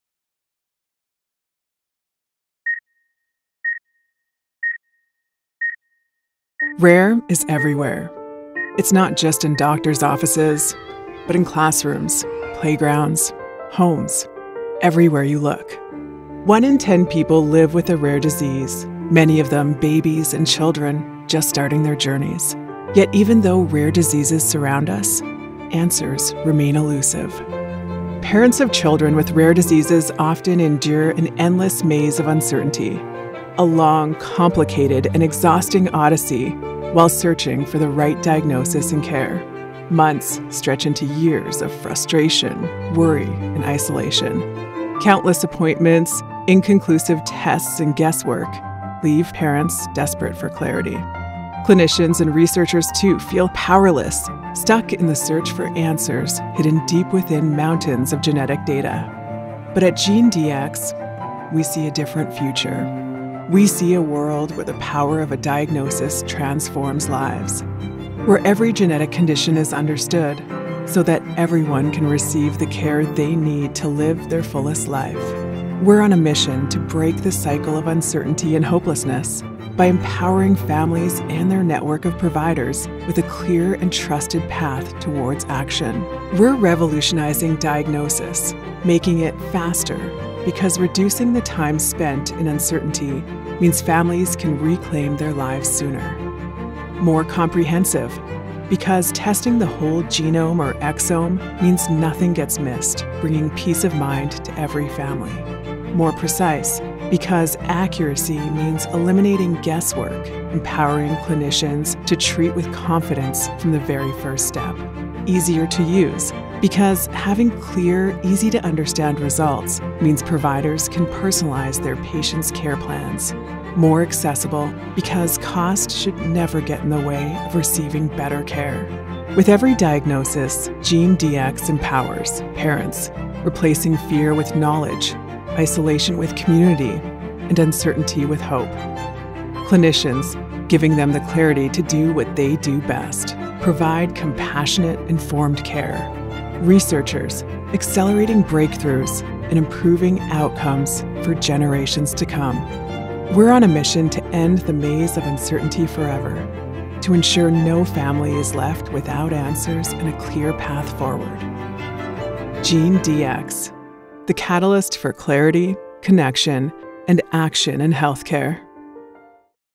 The Power of GeneDx | Our Manifesto
What If Genetic Answers Weren’t So Hard to Find?

For too long, families and clinicians have been left waiting—navigating a healthcare system that feels disconnected, overwhelming, and uncertain.

Genetic answers shouldn’t be rare or hard to reach. They should be clear, connected, and empowering - providing hope, direction, and real answers.

We're more than a leading genetic testing company, we're a partner to families, providers and researchers, giving them power through diagnosis.

Through advanced exome and whole genome sequencing, we bring clarity where there was confusion. We help families and clinicians move forward - with real, life-changing answers and clearer genetic diagnoses.

Because genetic testing is more than a test. It’s a turning point.

This film is a window into what happens when care becomes connected and answers become possible. It’s about ending the diagnostic odyssey—and giving people the power to see what’s next.

🎯 Our mission: To turn an overwhelming diagnostic journey into clear, actionable steps toward precision care

💡 Learn how we’re transforming fragmented genetic testing into a connected care journey